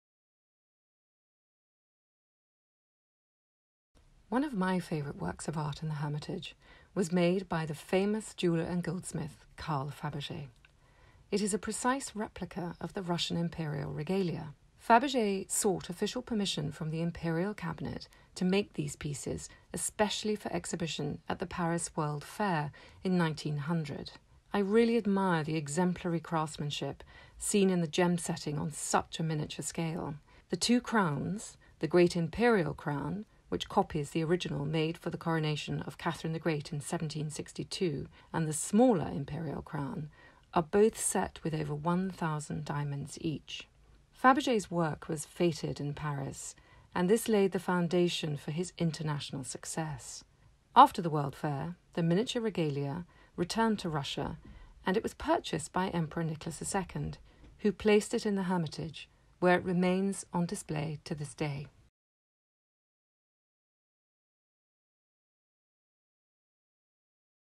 Carl Fabergé’s ‘Copy of The Imperial Regalia’ by Caroline de Guitaut
We asked our friends and supporters to pick a favourite work of art from The State Hermitage Museum collections and to explain why.  Here is Caroline de Guitaut, Trustee and Deputy Surveyor of The Queen's Works of Art, speaking about Carl Fabergé’s ‘Copy of The Imperial Regalia’. This film is part of the My Favourite Work campaign on Instagram and Youtube. Video made by Hermitage Foundation UK in support of The State Hermitage Museum, St Petersburg.